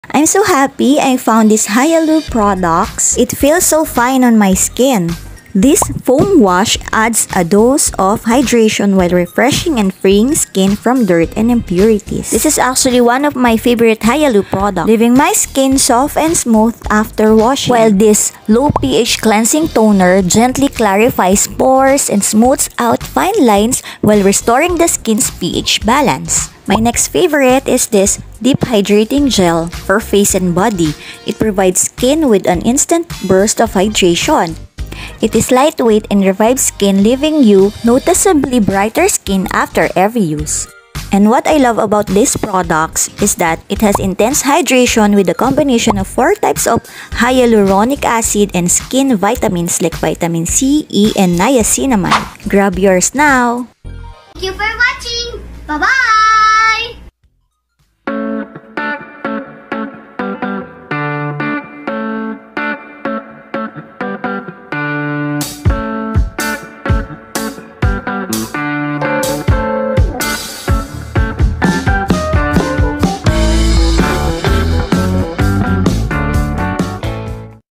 KUNG SENSITIVE AND DRY SKIN KA TRY MO TO| HYALOO PRODUCT REVIEW|I'm a Happy Mom Ph
Sensitive Skin??
In this video I will share with you the skincare product that I'm using. Have you heard about Hyaloo product? Let me introduce it to you keep watching!

If you're reading this, I want you to know that God loves you!